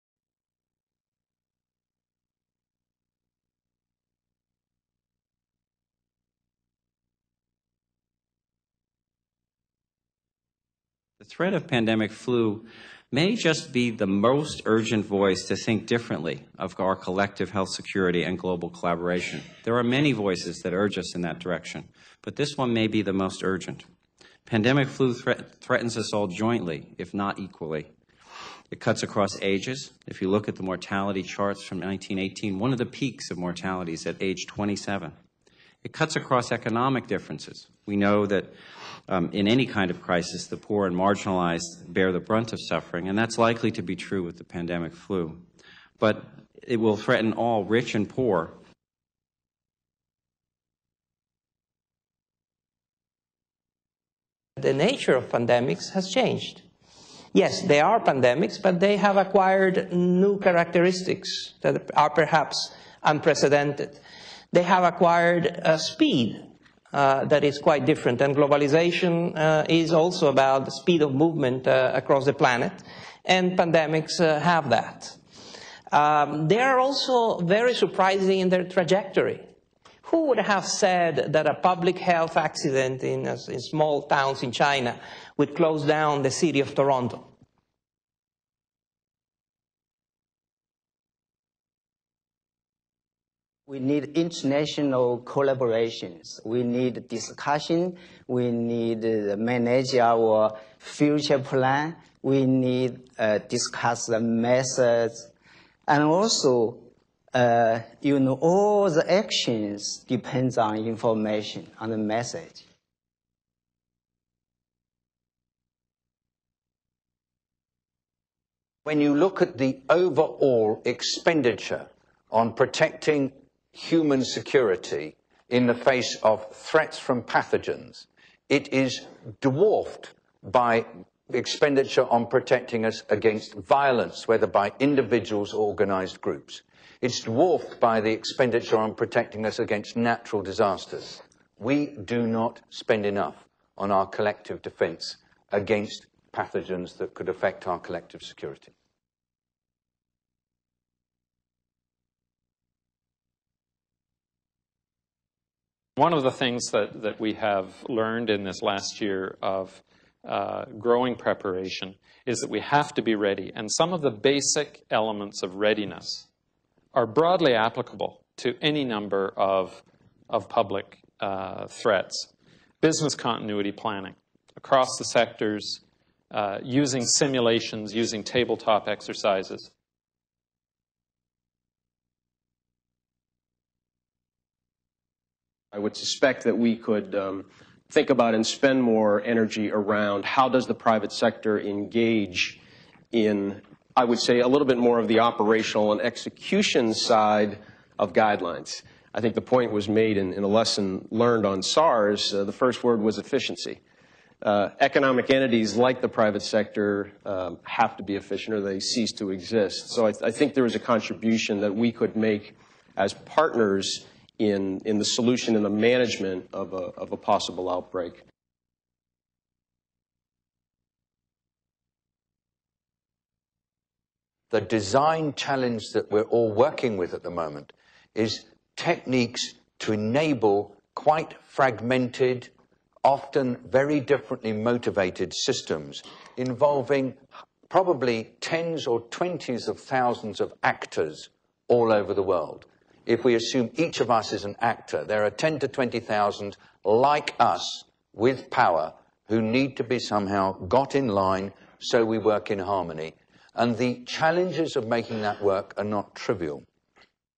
Plenary 3 - How do we prepare for the next Pandemic? - 2006 Pacific Health Summit
Plenary Session 3 "How do we prepare for the next pandemic?" at the 2006 Pacific Health Summit, held in Seattle in June 2006. This session features: Chris Elias, Moises Naim, Depei Liu, David Nabarro, Ian Shugart, and Mark Vachon.